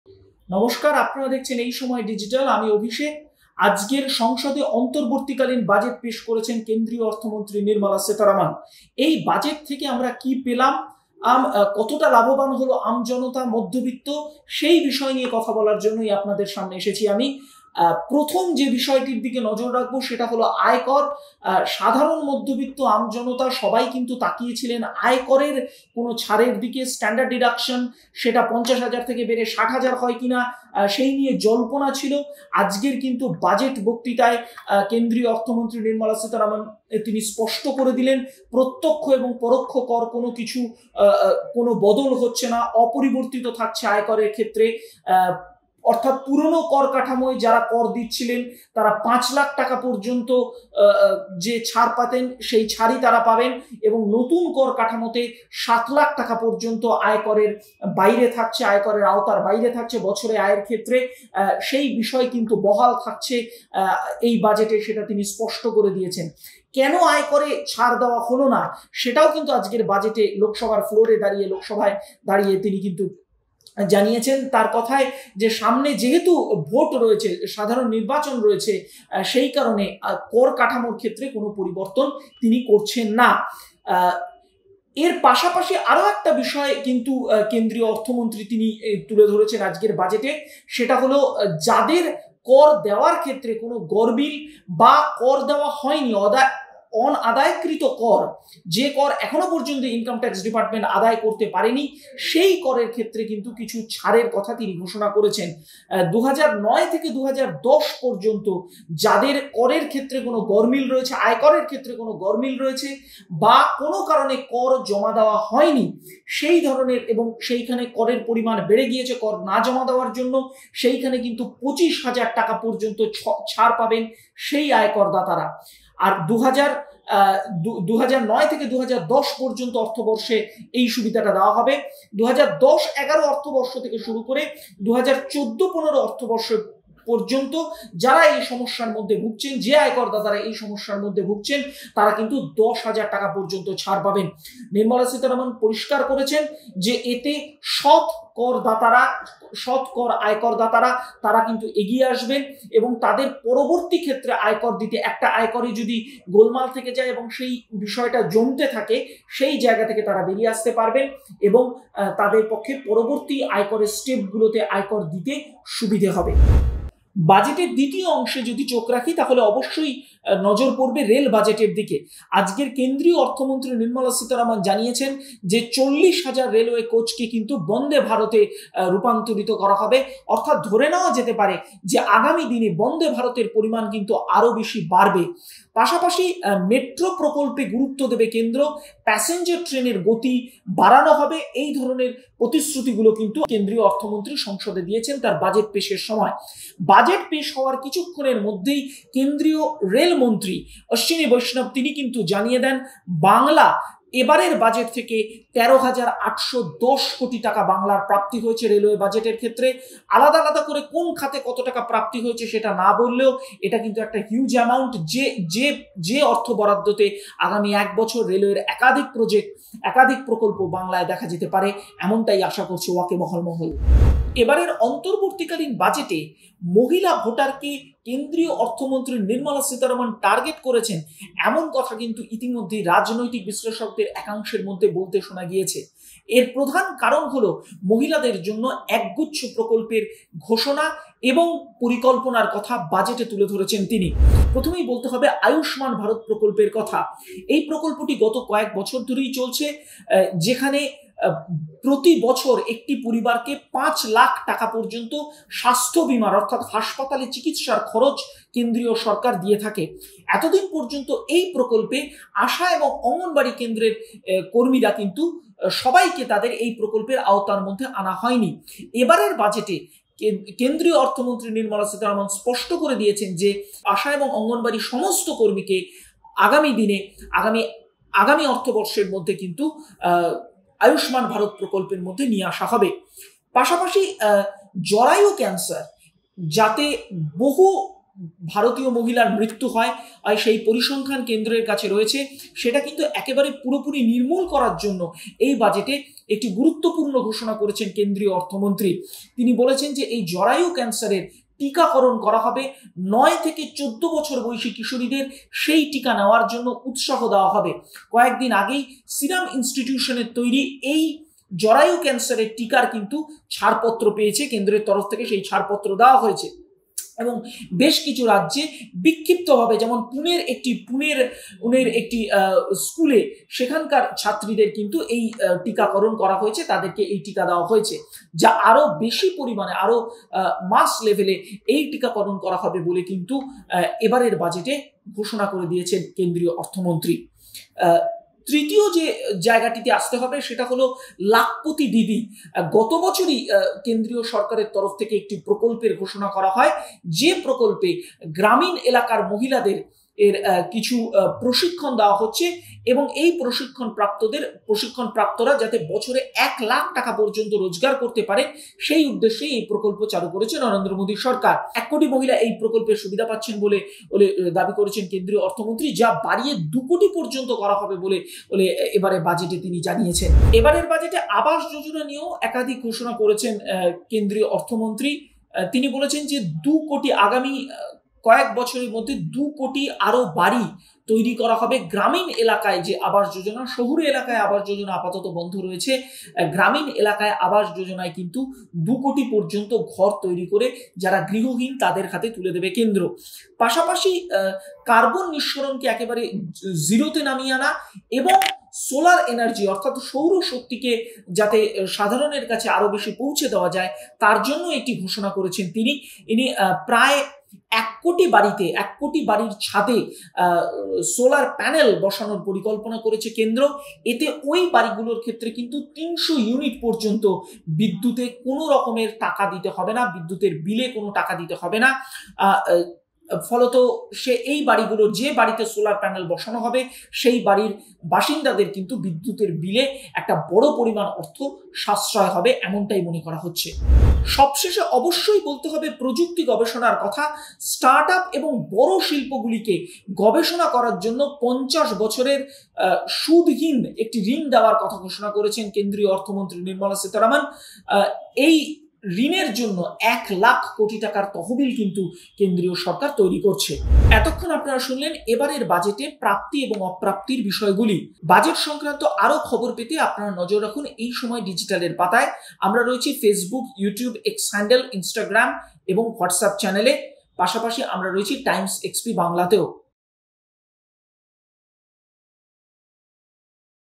Union Budget 2024 : বাজেটে আয়করে আদৌ স্বস্তি পেল মধ্যবিত্ত? | Nirmala Sitharaman | Ei Samay
বাজেট অধিবেশন হয়ে গেল গতকাল। বাজেটে আয়করে কী কী স্বস্তি পেল মধ্যবিত্ত? সেই বিষয়ে আলোচনা করা হল এই ভিডিয়োতে। আয়করে গরমিলে ২৫ হাজার টাকা ছাড়  দেওয়া হয়েছে। বাংলায় রেলে ১৩,৮১০ কোটি টাকা বরাদ্দ করা হয়েছে। আশা-অঙ্গনওয়াড়ি কর্মীদের জন্য আয়ুষ্মান-প্রতিশ্রুতি। জরায়ু ক্যান্সারে টিকাকরণের ঘোষণা করা হল। সৌর বিদ্যুতে 300 ইউনিট পর্যন্ত ফ্রি ঘোষণা করা হয়েছে।